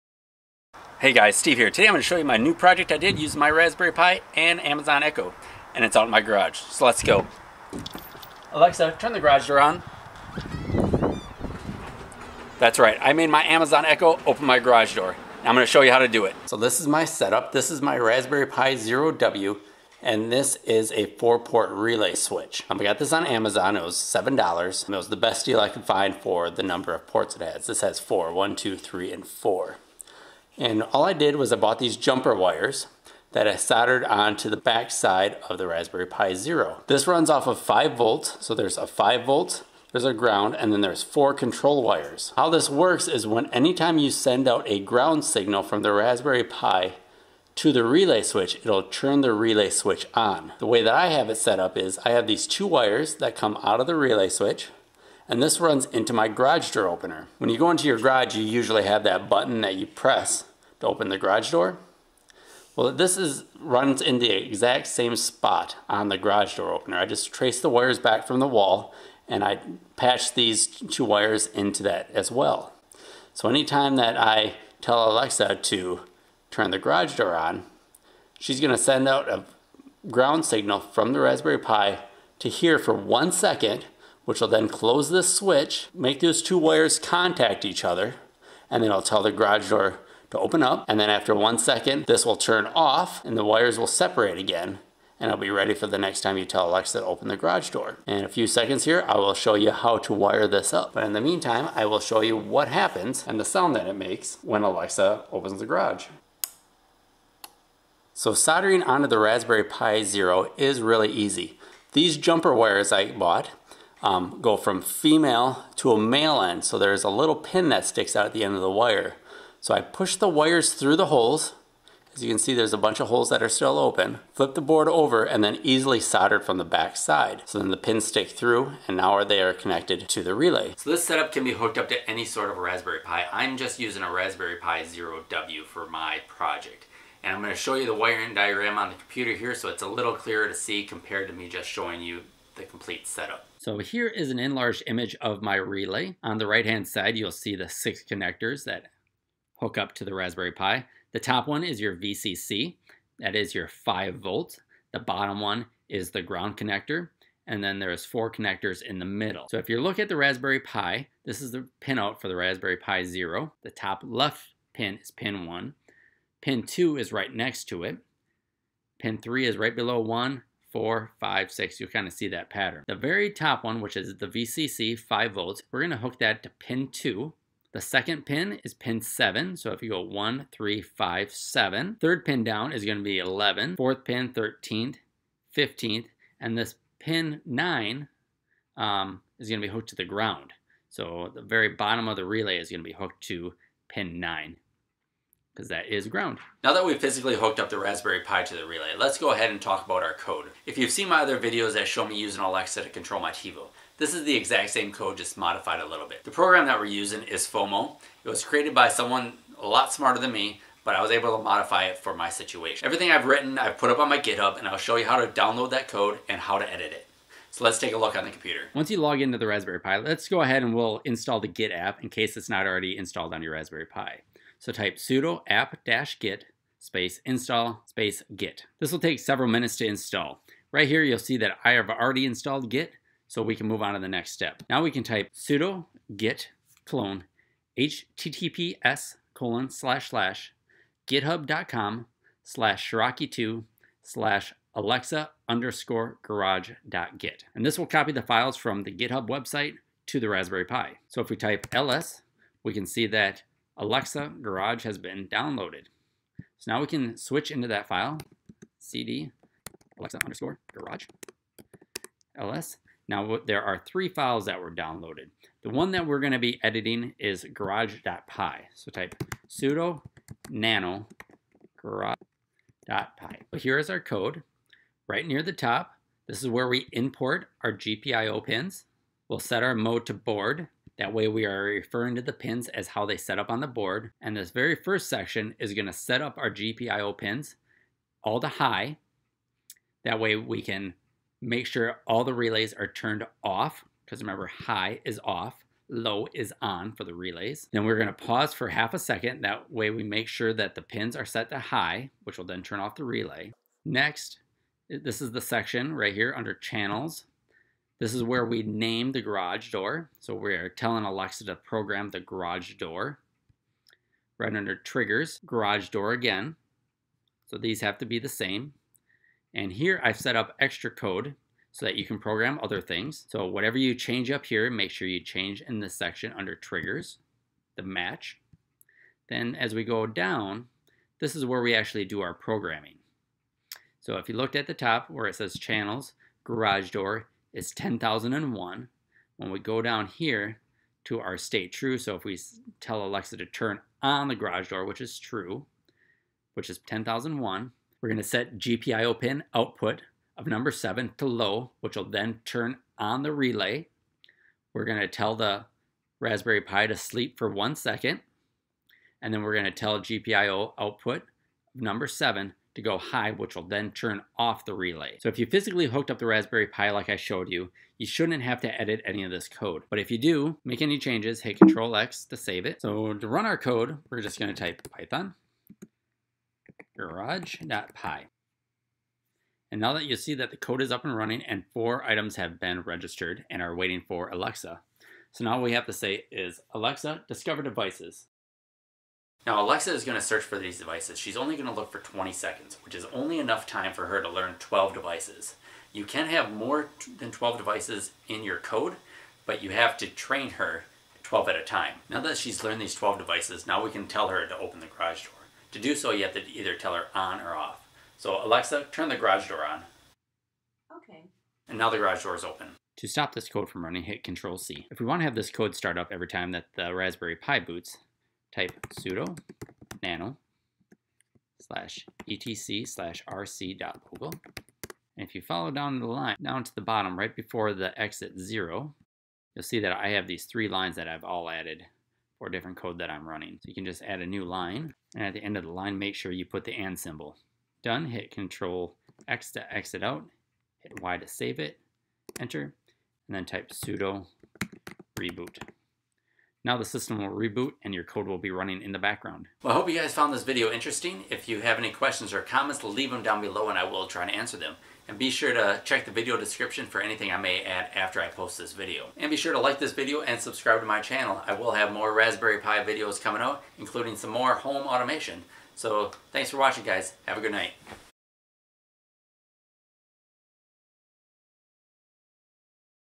Alexa Controlled Garage Door Opener Using Raspberry Pi
PLEASE NOTE: recent firmware changes have caused this project to only work if a 2nd Generation Echo Dot ) is on your network. You can use other Echos as long as a 2nd Gen Echo Dot is on the network too.

Using Alexa on the Amazon Echo to open my garage door. Requires programing your Raspberry Pi & a Relay Switch.

To Install GitHub software, type:
sudo apt-get install git

To run code, type:
python garage.py